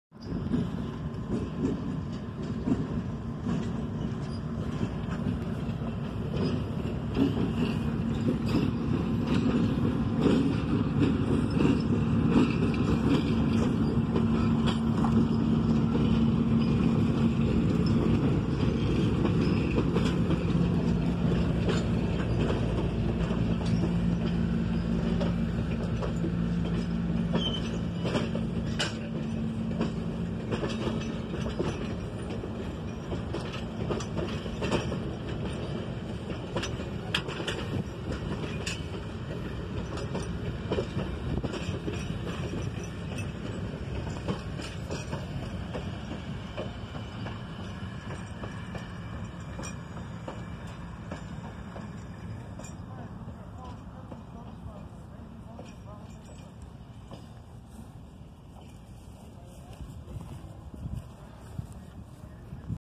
The Rocktin Rion & Western Christmas Train chase part 2 @ Winsburo S.C. 12/21/19.
Rocktin Rion & Western Train with locomotive 2015 Headed Westbound on the RR&W Railroad mainline in winsburo S.C. I really hope y'all enjoy the video.